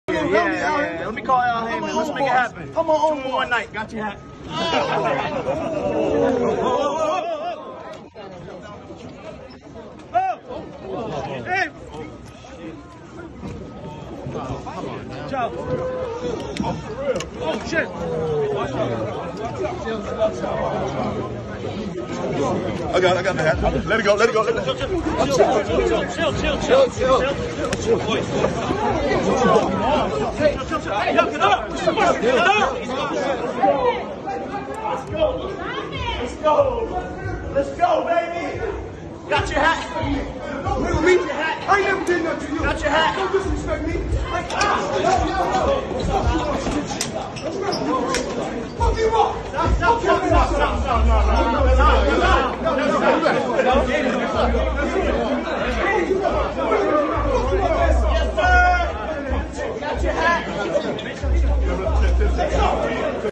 Jake Paul le quita la gorra a FLOYD MAYWEATHER y todo se complica en Miami
Todo se veía falso. Cada palabra, cada gesto, cada intento de lucir genuino. Logan Paul y Floyd Mayweather estaban en su elemento cuando el jueves intentaron vender su pelea del 6 de junio en el Hard Rock Stadium. Se hicieron pasar por benefectores del boxeo cuando en realidad lo están saqueando.
Todo estaba escrito en el guión. Ambos se presentaron delante de la prensa. El youtuber con su aire de descaro, tratando de demostrar cuanto había avanzado en su práctica de un deporte milenario, Mayweather intentando hacer creer que su espíritu de guerrero estaba de vuelta.
Pero incluso dentro del guión algo se escapó de las manos. Cuando Jake Paul, hermano menor de Logan e igualmente saqueador de cuadriláteros, le quitó la gorra a Mayweather, algún resorte saltó dentro del verdadero peleador que le encendió la sangre.